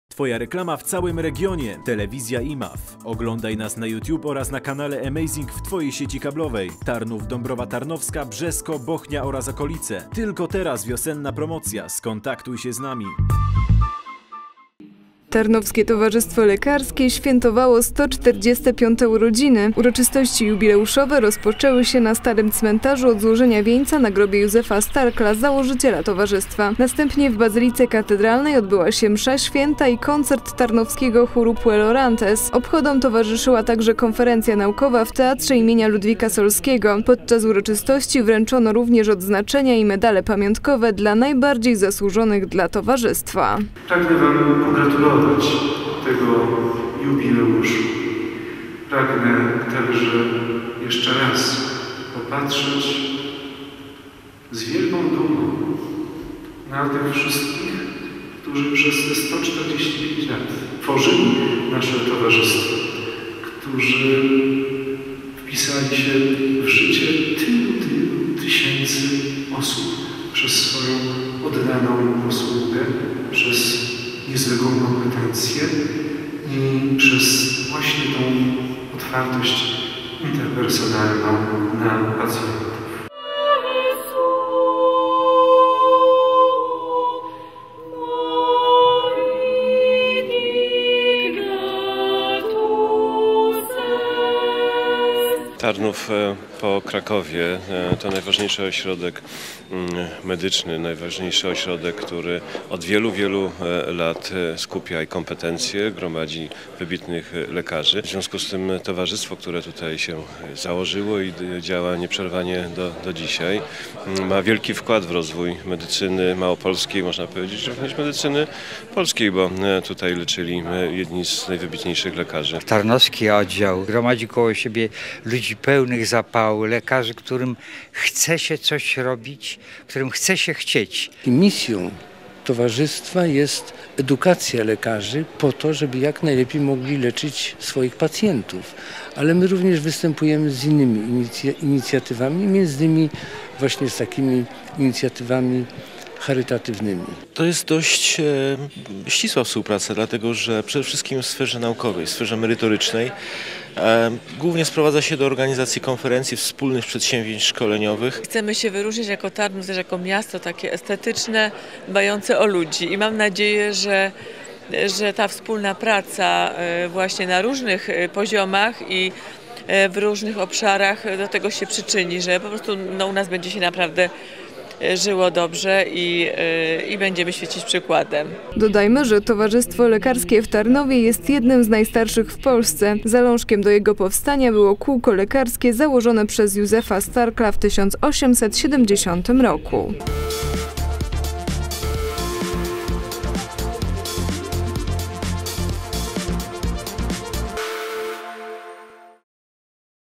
Tarnowskie Towarzystwo Lekarskie świętowało 145-urodziny - Magazyn Miejski 20/04/2015 - imav.tv
Tarnowskie Towarzystwo Lekarskie świętowało  145-urodziny. Uroczystości jubileuszowe rozpoczęły się na Starym Cmentarzu  od złożenia wieńca na grobie Józefa Starkla, założyciela towarzystwa. Następnie w Bazylice Katedralnej odbyła się msza święta i koncert tarnowskiego chóru Puellae Orantes.  Obchodom towarzyszyła konferencja naukowa w teatrze im. Ludwika Solskiego. Podczas uroczystości wręczono także odznaczenia i medale pamiątkowe dla najbardziej zasłużonych  dla towarzystwa.